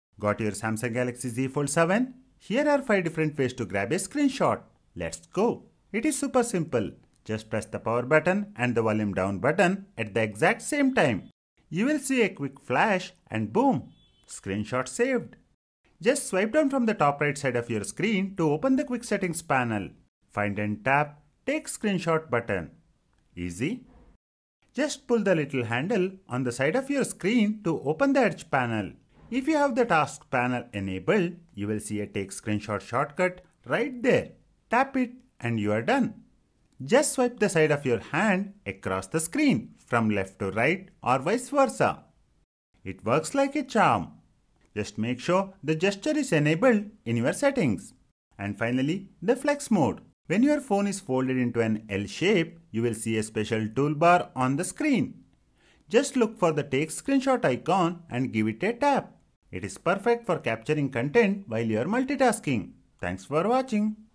Samsung Galaxy Z Fold 7: 5 Ways to Screenshot!
Short video on how to take screenshot using 5 different methods.